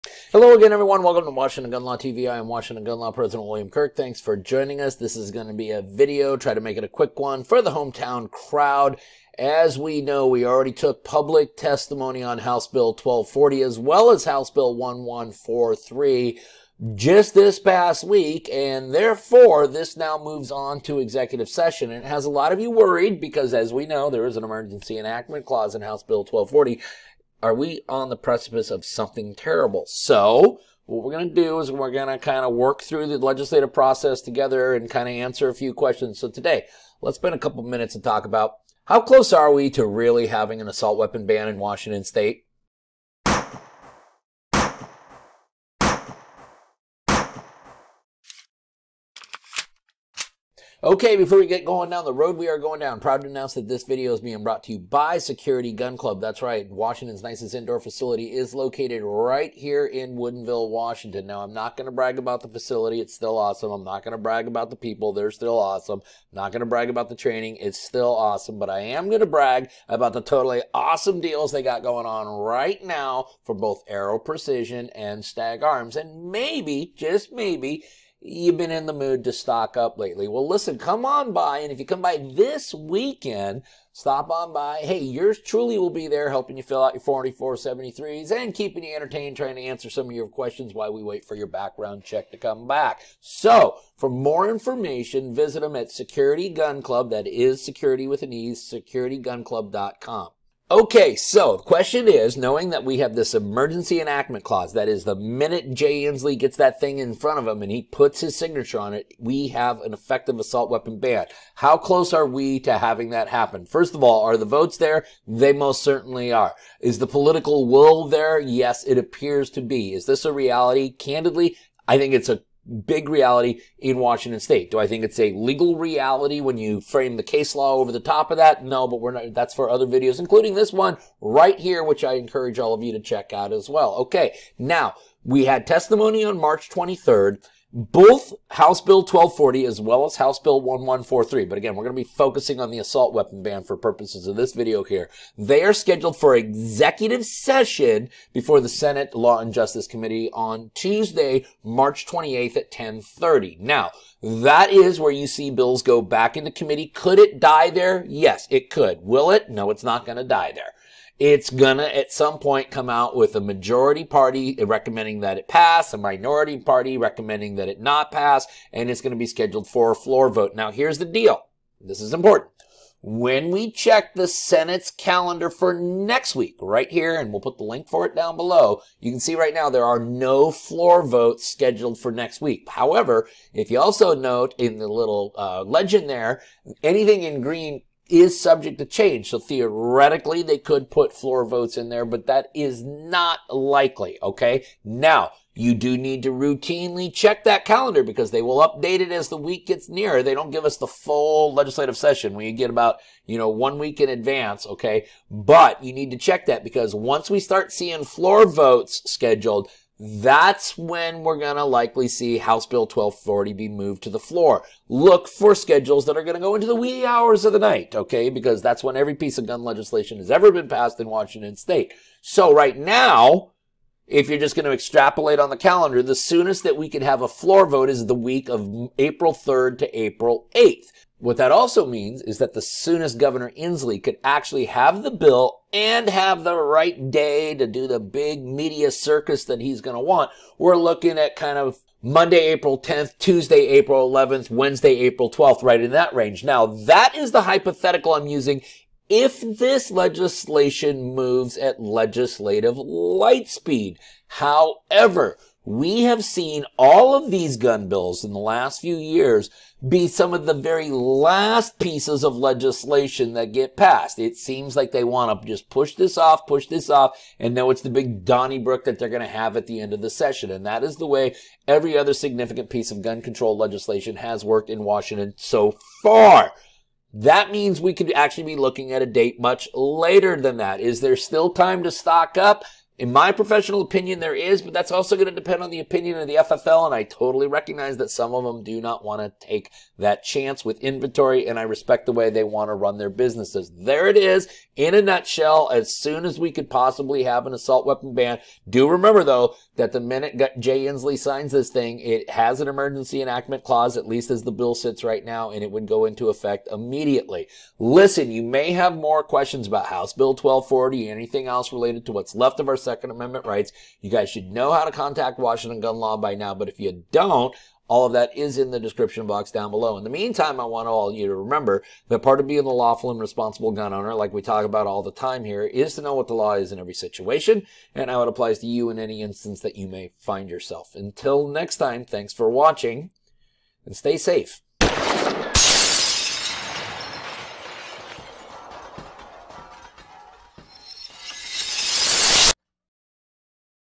How Close Are We To Really Having an Assault Weapon Ban in Washington State?
Washington Gun Law President, William Kirk, takes a look at the upcoming legislative calendar to try to determine when is the soonest that HB 1240, Washington' 7th attempt at an assault weapon ban might actually be put to a floor vote.  Learn the answers to some of these very important questions and arm yourself with education today.
Stay safe!!!